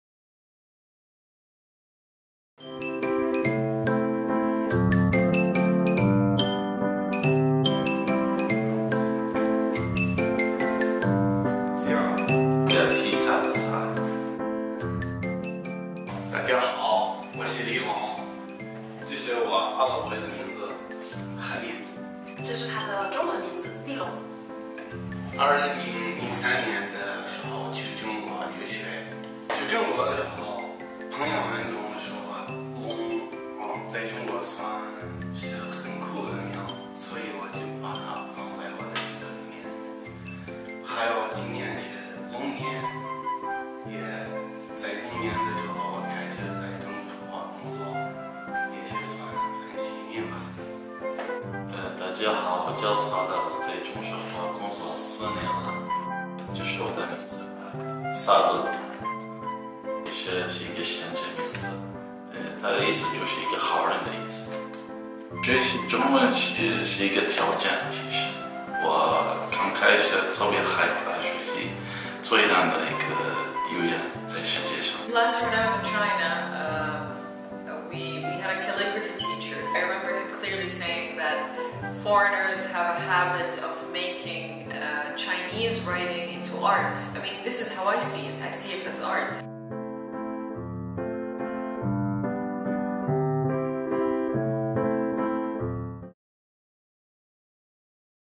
The Story Of My Chinese Name
✍On ChineseLanguageDay, Sinopec employees wrote down their Chinese names and shared heartwarming stories behind their names.

Li Loong explained why he chose the character "Loong" (Chinese Dragon) for his Chinese name. Saleh mentioned that his Chinese name is derived from a title of a prophet, symbolizing kindness and integrity. And Sarah believed that Chinese characters are an art form, full of charm and depth.

🌟Click the video to discover their stories behind their Chinese names and experience the amazing cultural and emotional resonance!